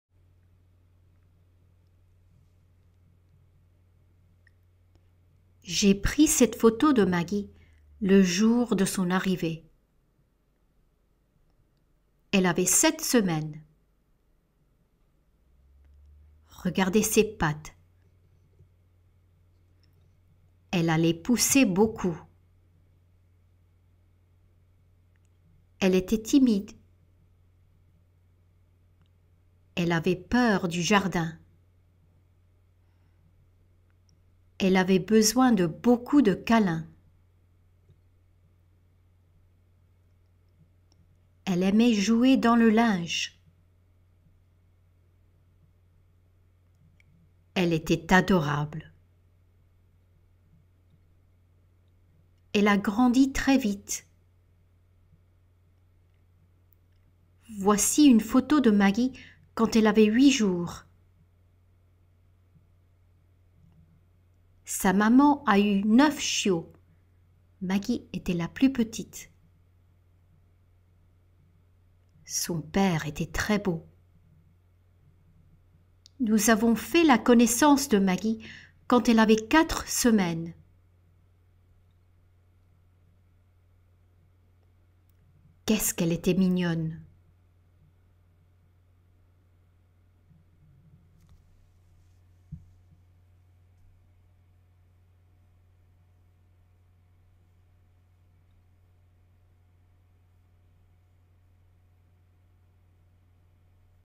Imperfect vs Perfect tense in French in a short story
Imperfect tense in French verb practice with free worksheet + quiz Short sentences about a new puppy using imperfect vs passé composé + Avoir and Être practice. A Green Mouse French